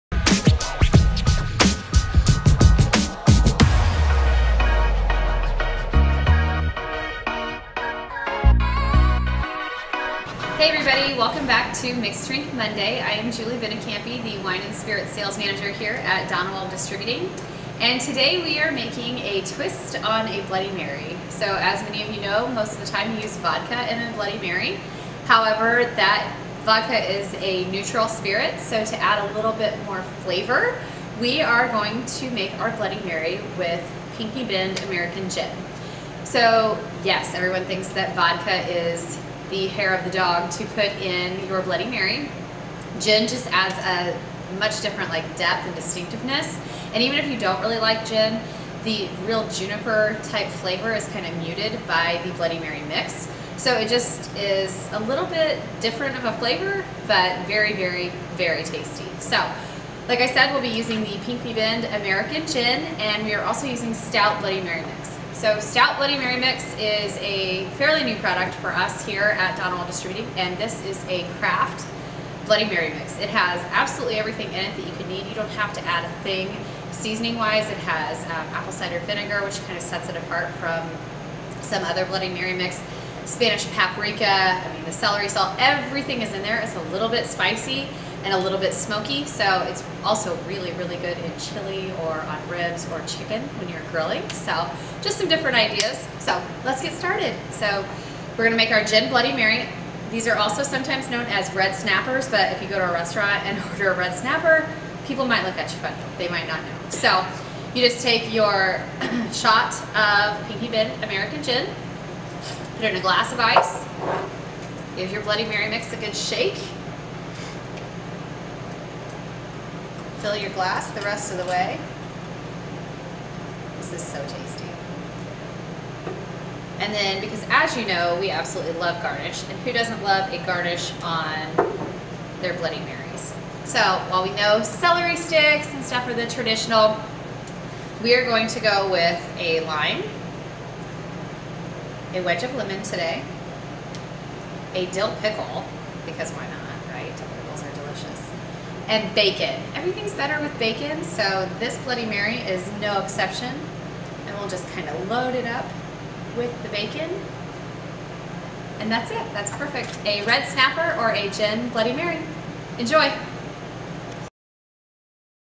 Mixed Drink Monday- Gin Bloody Mary
This Mixed Drink Monday learn how to make a Gin Bloody Mary!

Step 1: Pour 1oz of Pinckney Bend American Gin into a glass with ice.
Step 2: Fill the rest of the glass with STOUT Bloody Mary Mix.
Step 3: Garnish with your favorite toppings (we used bacon, limes, lemons, and a dill pickle for this Bloody Mary).